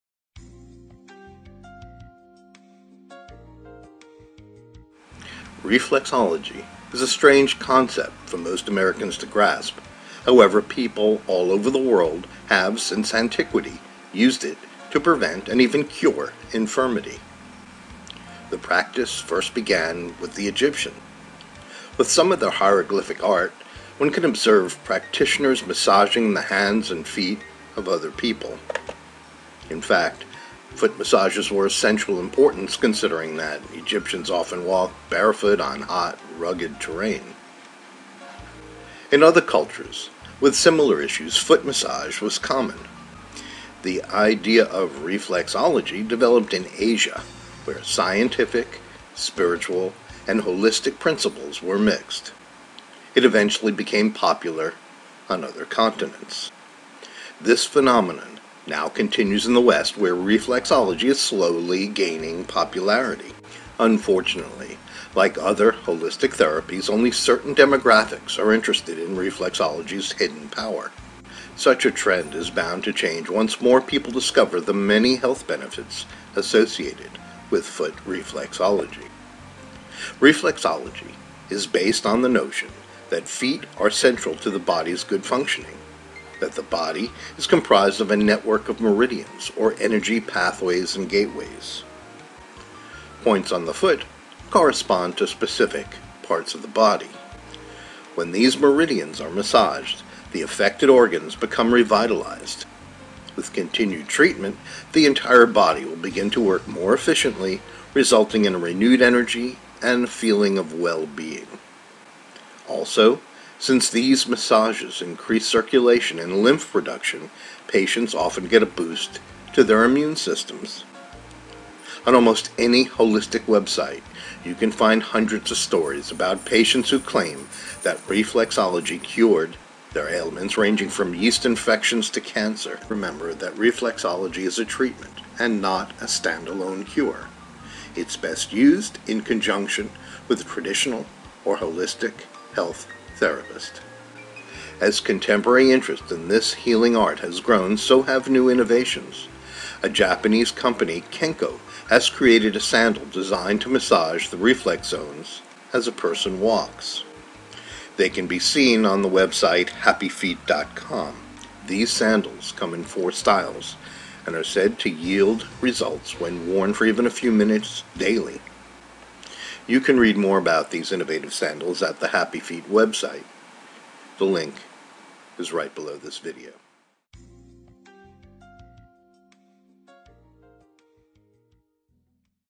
Health Benefits of Reflexology
From a simple foot massage to a holistic treatment for a variety of ailments, Reflexology is becoming recognized, even in the West, as having health benefits.